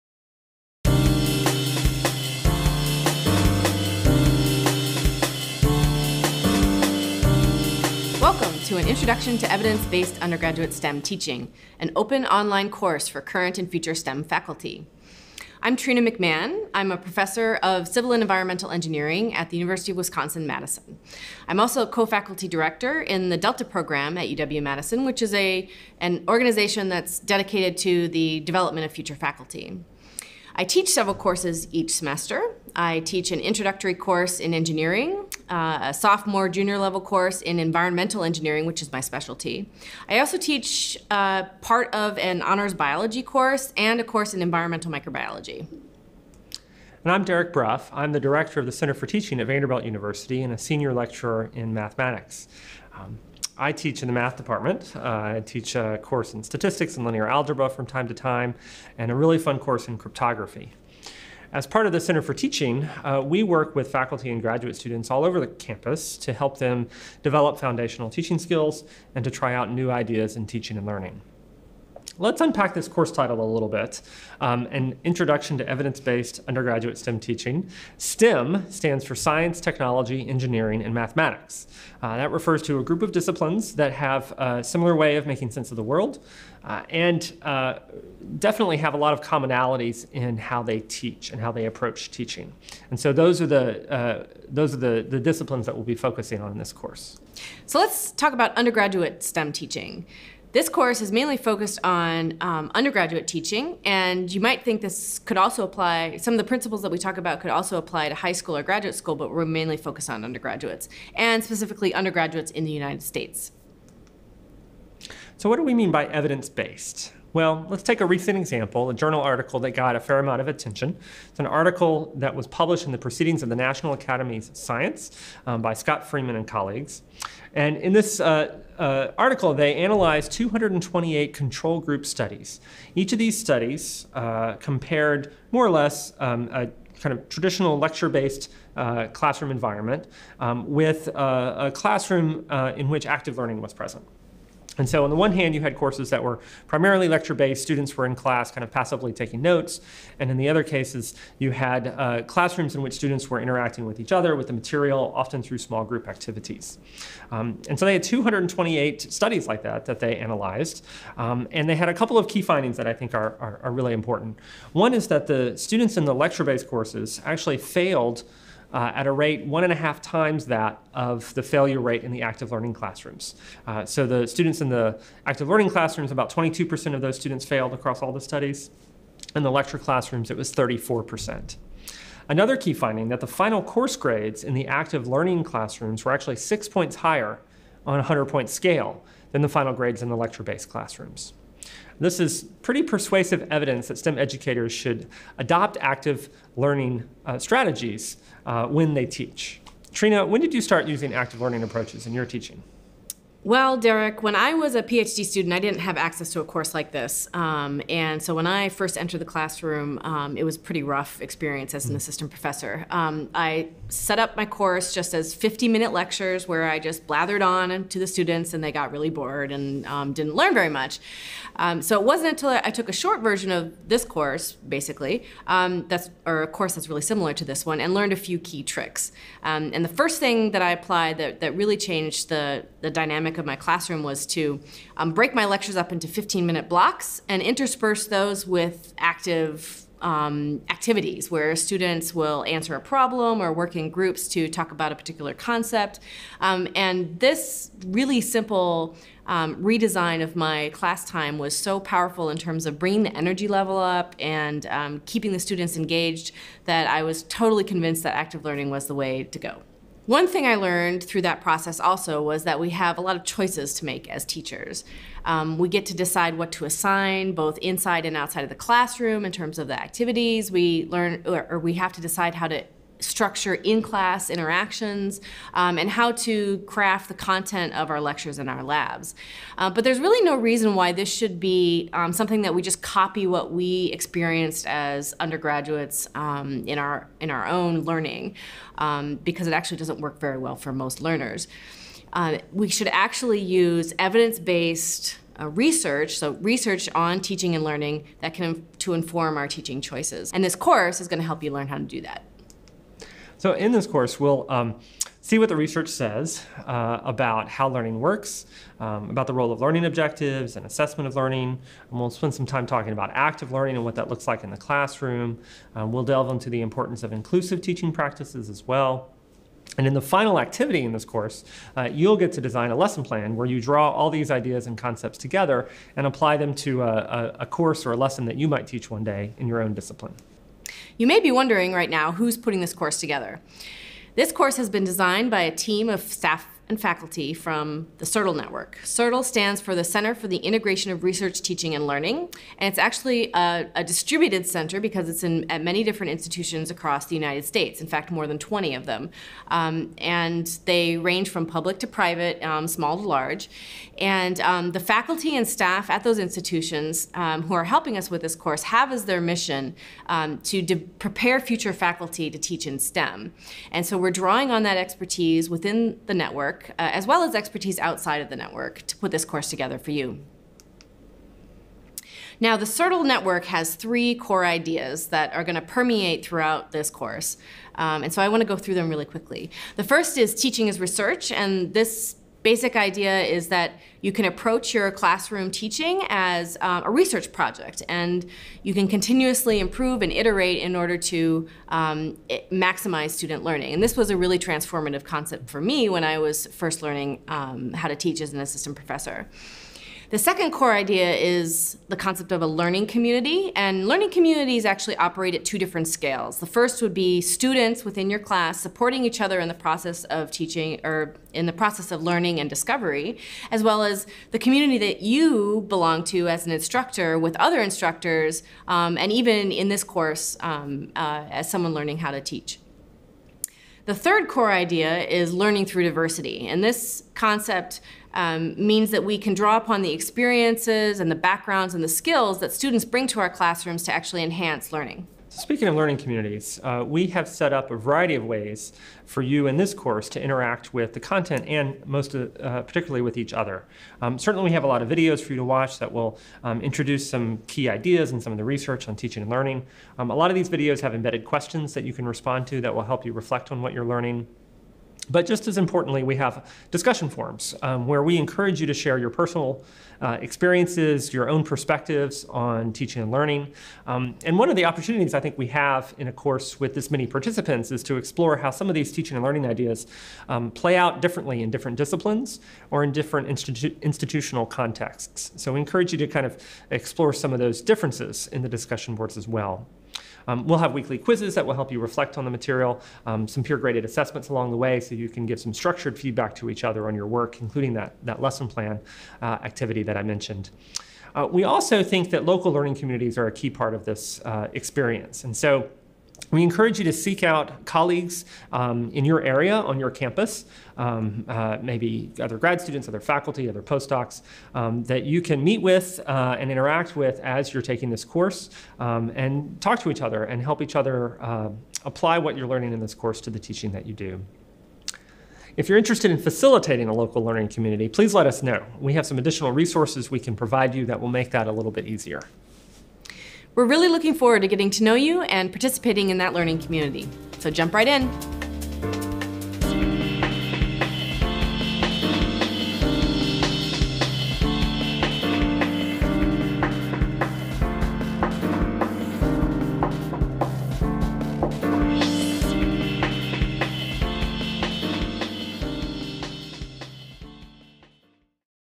An Introduction to Evidence-Based Undergraduate STEM Teaching | BUx on edX | Course About Video
Learn how to implement evidence-based teaching strategies in your university classroom as well as effective methods for assessing teaching and learning.

This course is designed to provide future STEM faculty, graduate students and post-doctoral fellows with an introduction to effective teaching strategies and the research that supports them.

The goal of the eight-week course is to equip the next generation of STEM faculty to be effective teachers, thus improving the learning experience for the thousands of students they will teach.

The course draws on the expertise of experienced STEM faculty, educational researchers, and staff from university teaching centers, many of them affiliated with the Center for the Integration of Research, Teaching, and Learning (CIRTL), a network of research universities collaborating in the preparation of STEM graduate students and post-docs as future faculty members.

- Key learning principles such as the role of mental models in learning and the importance of practice and feedback
- Fundamental elements of course design, including the development of learning objectives and assessments of learning aligned with those objectives
- Teaching strategies for fostering active learning and inclusive classroom environments